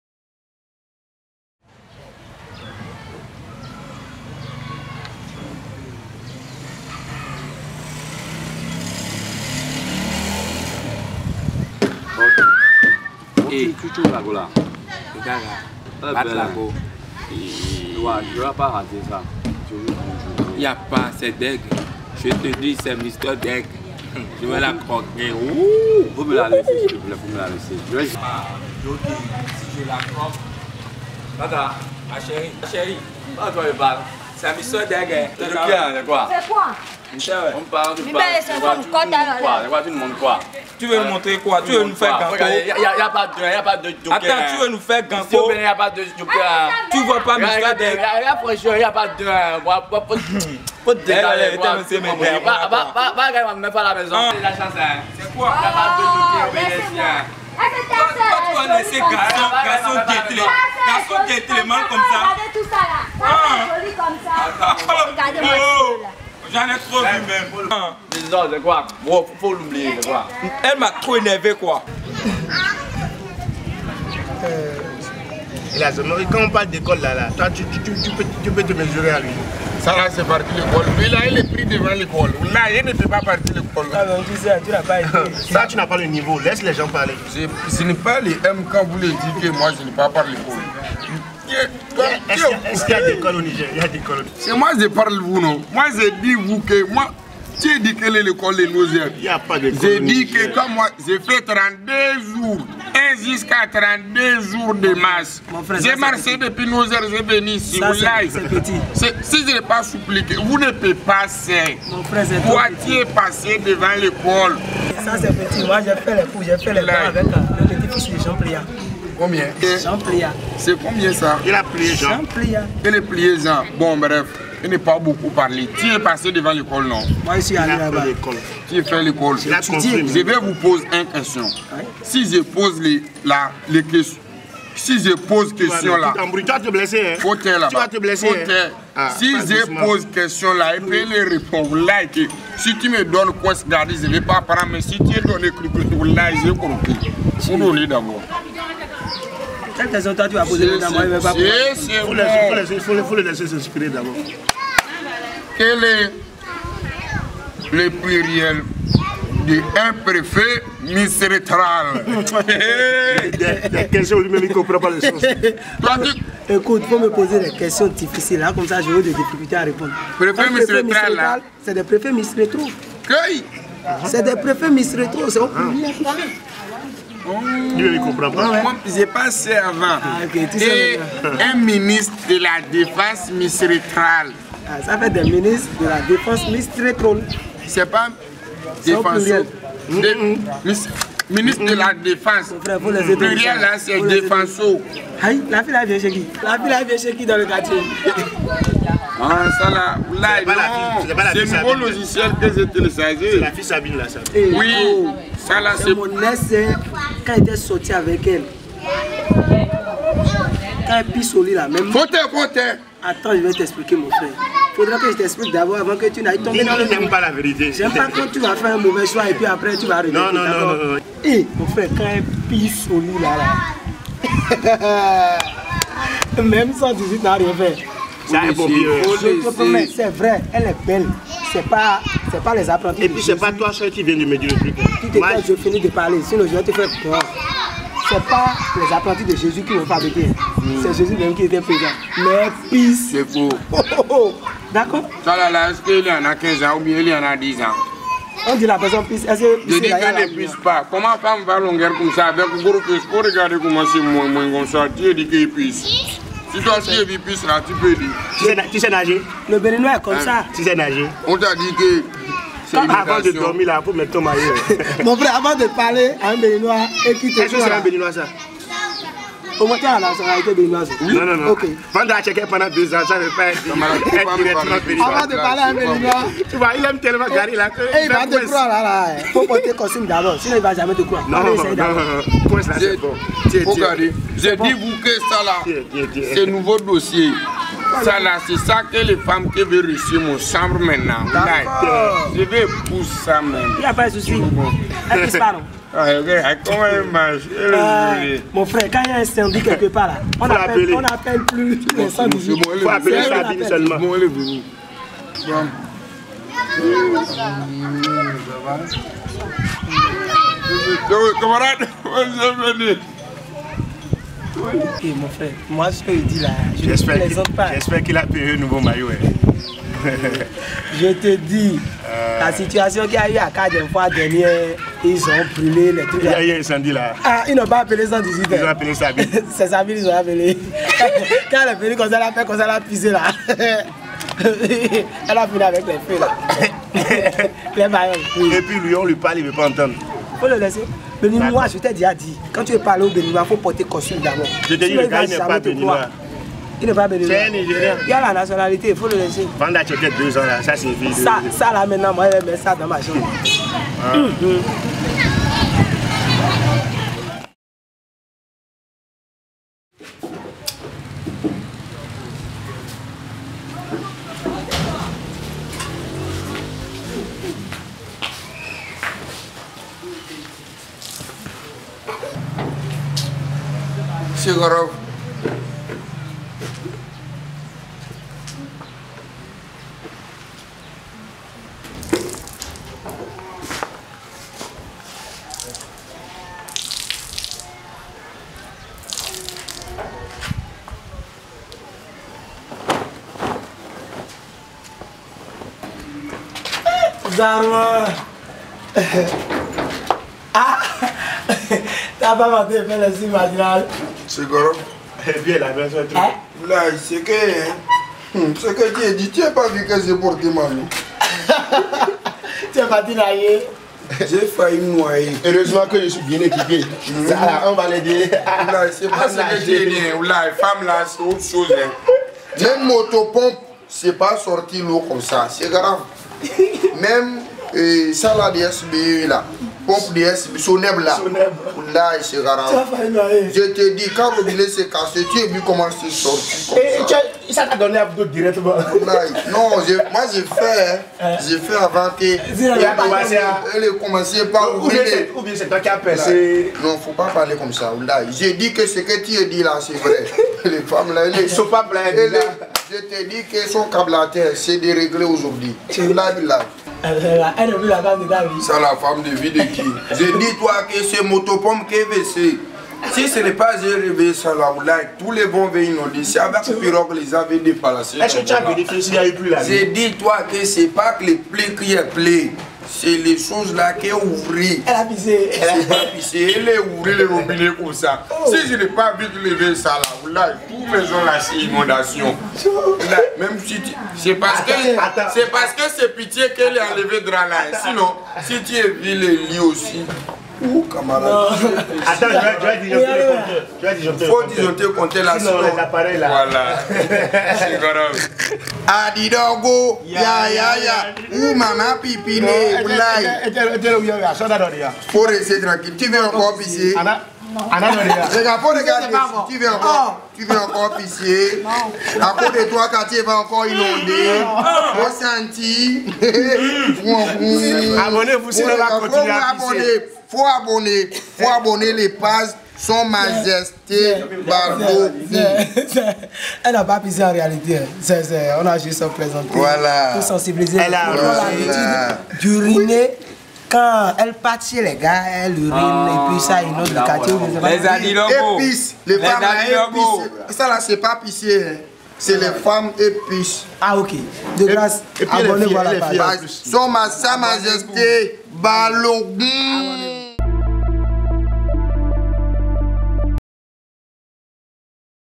Femme Robinet du quartier - Sa Majesté Balogoun
Compagnie Balogoun, un groupe d’humoristes dans femme pisseuse, la femme robinet, c'es pas une realité mais juste pour faire rire nos fans.